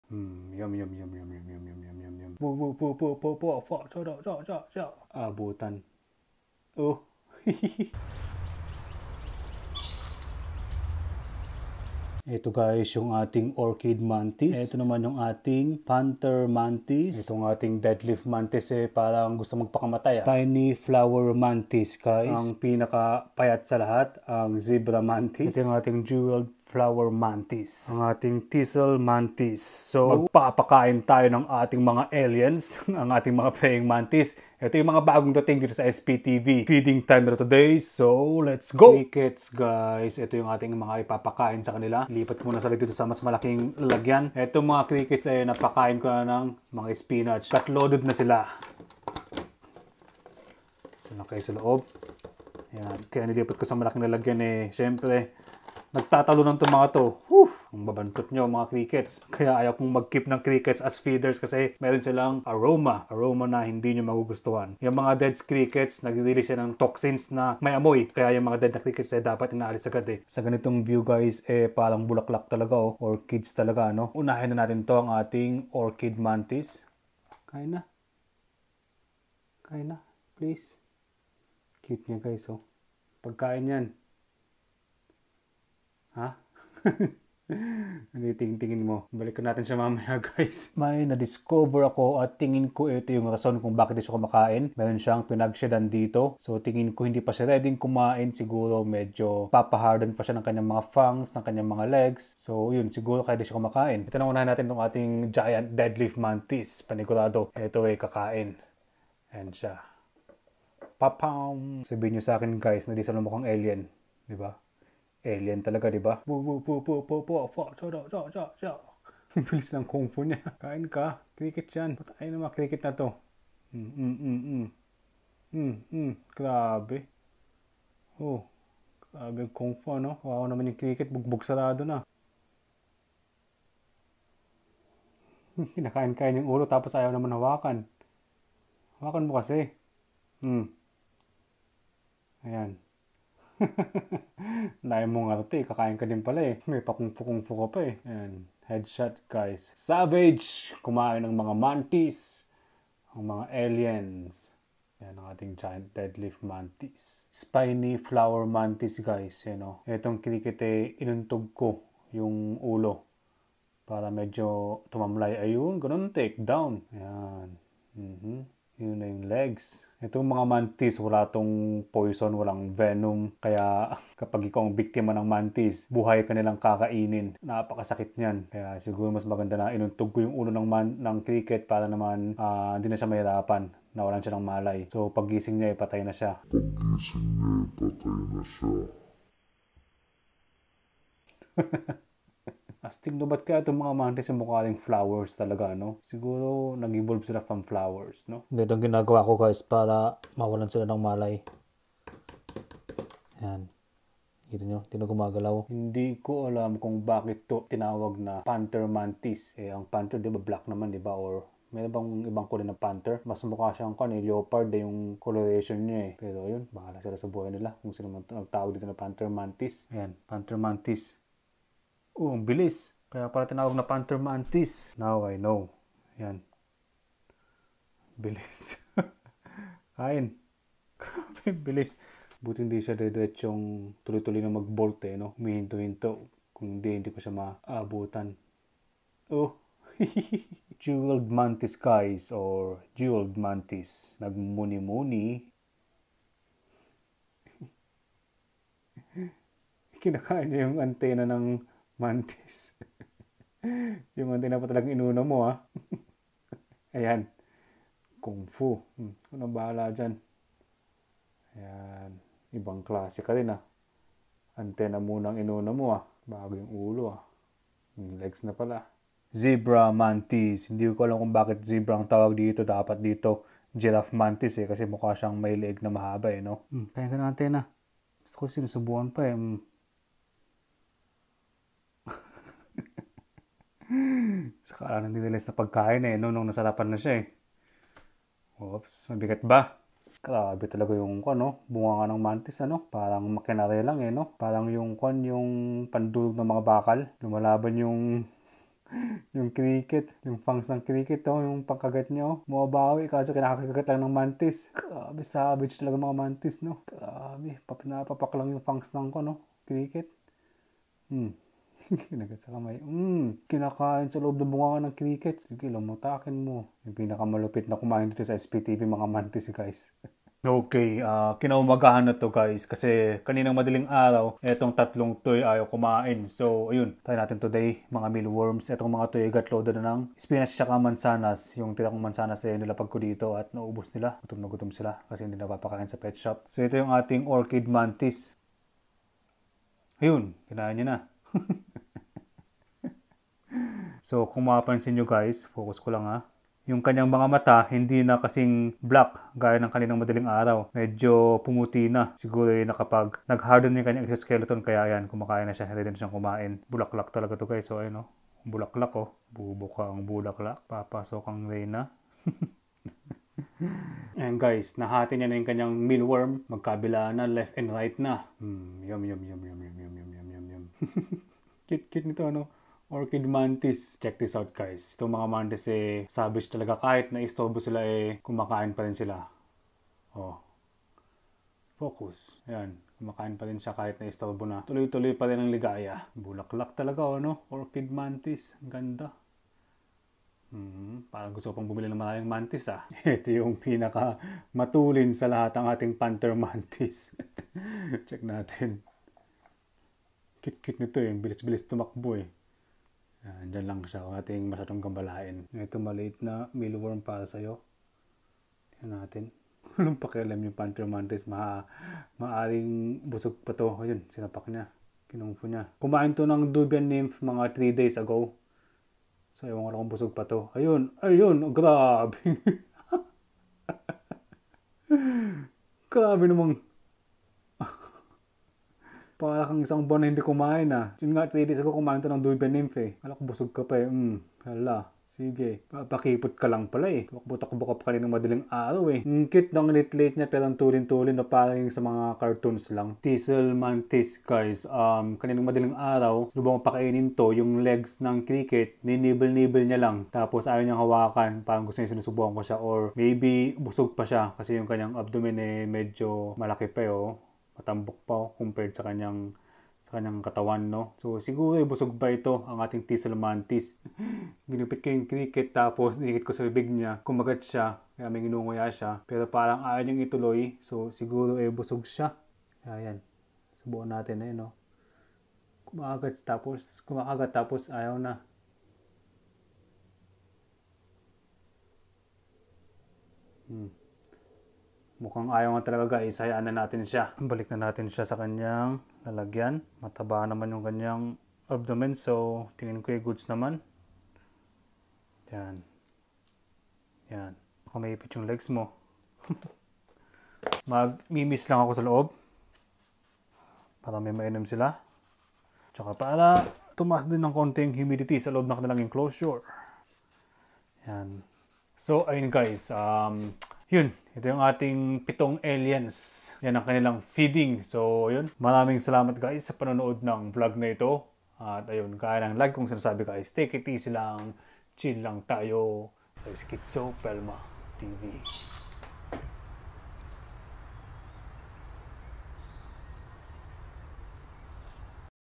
Feeding all my pet mantis - Laughtrip ito!
Astig na nga.. Laughtrip pa.. Nakakatuwa mag-pakain ng mga praying mantis.. May lutang.. may nagbo-bolt.. may kung-fu master.. Pili na kayo sino mas cool.. haha

Enjoy lang as always at take it easy lang guys.. Maraming salamat!!
Sha-ring is caring guys...